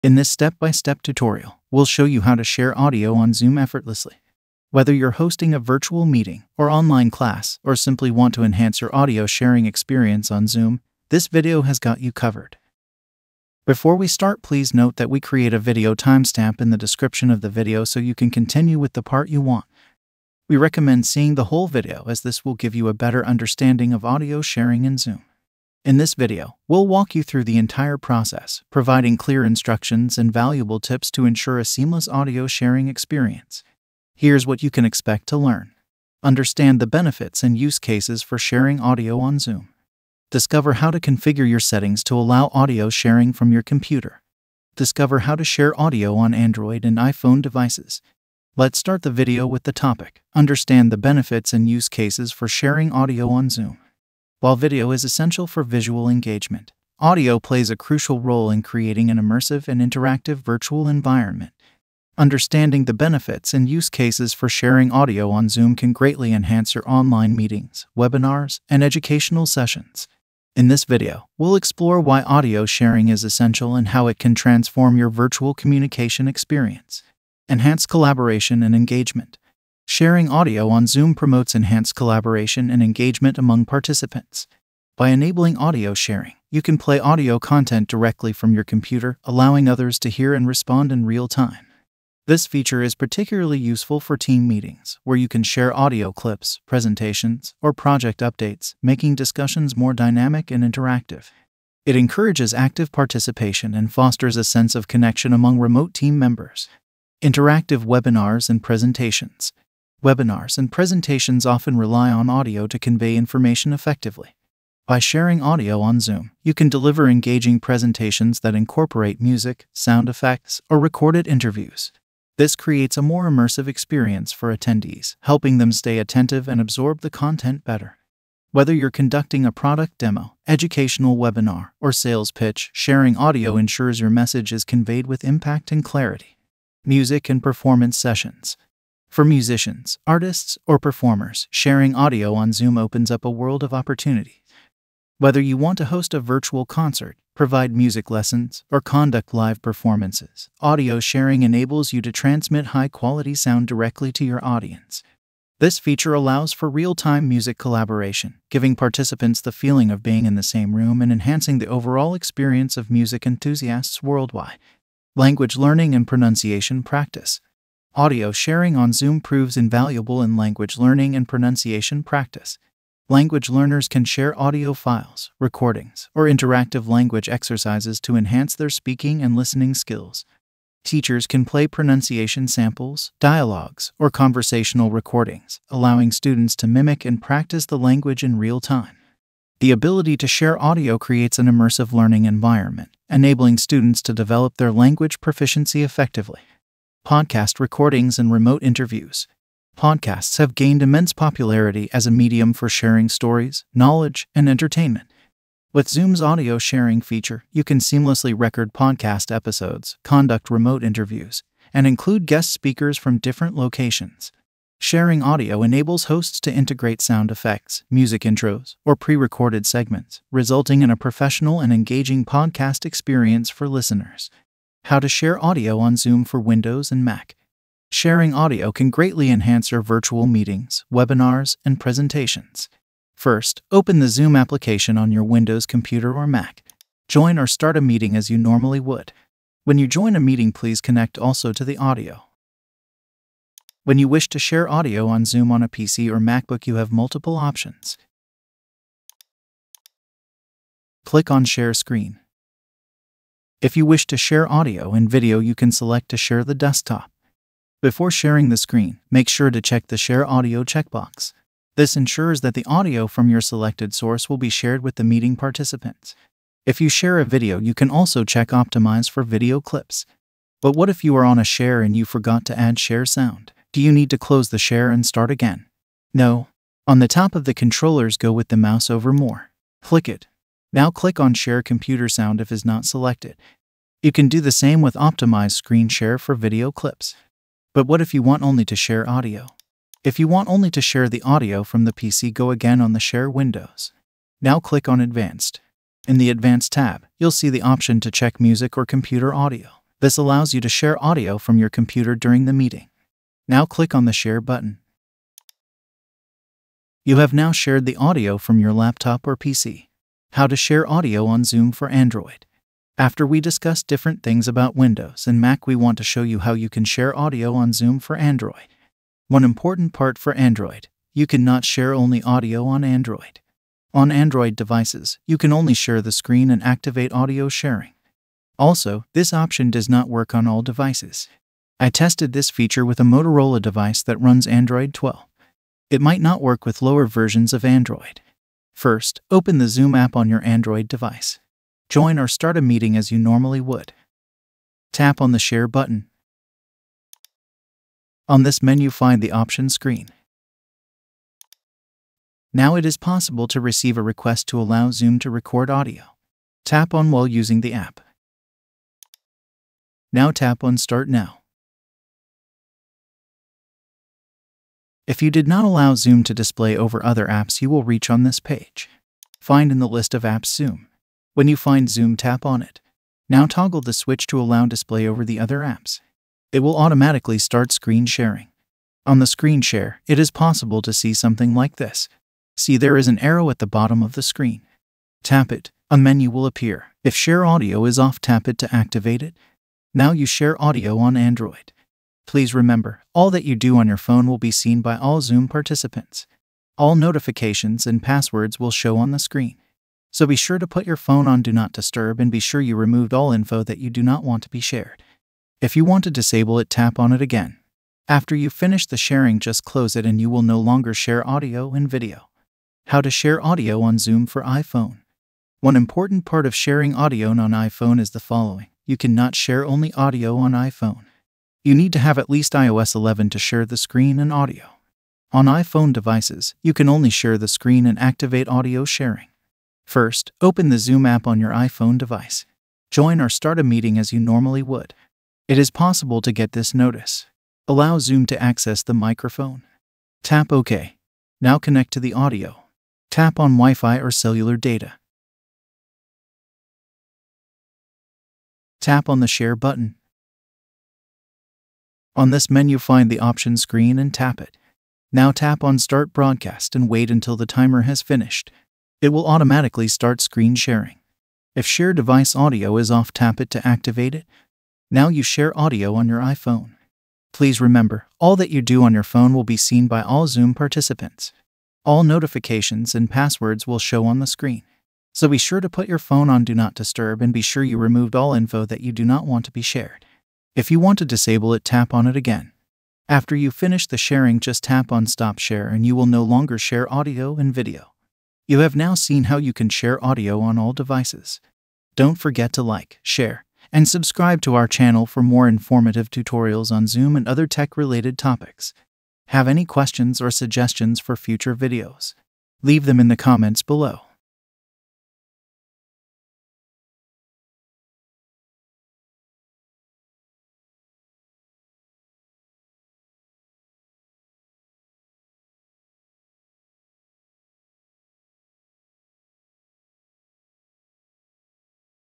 How to Share Audio on Zoom
We'll show you how to share audio on Zoom in this step-by-step tutorial. This video will help you whether you are having a virtual meeting, an online class, or just want to improve the way you share audio on Zoom.

Sharing sounds on Zoom can make it much easier for people to work together, get involved, and talk to each other during online meetings. But many users don't know how it works and often have trouble setting up music sharing in a good way. With our help, you'll be able to use this tool like a pro in no time.

Timestamps:
0:00 Introduction
0:56 Understand the benefits and use cases for sharing audio on Zoom.
3:50 How to Share Audio on Zoom on Windows and Mac
5:16 How to Share Audio on Zoom on Android
6:59 How to Share Audio on Zoom on iPhone